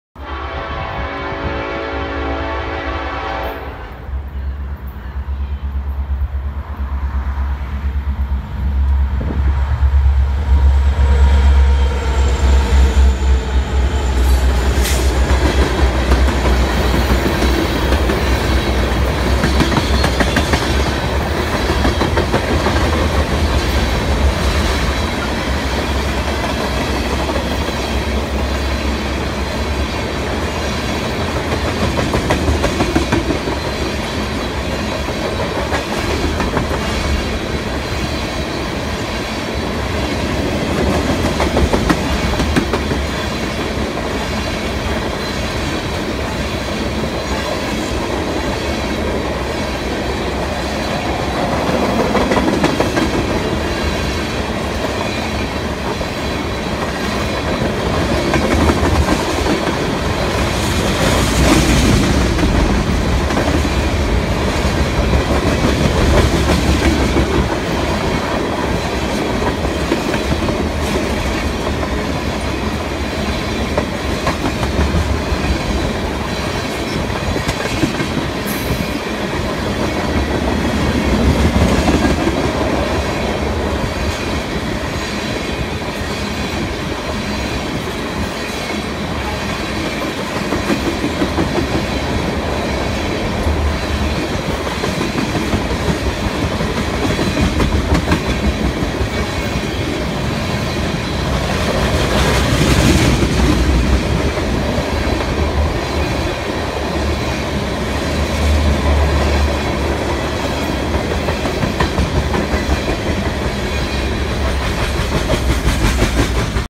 NS Westbound Jersey Central Trailing
NS Westbound with Jersey Central Trailing Cove PA Jan 1st 2019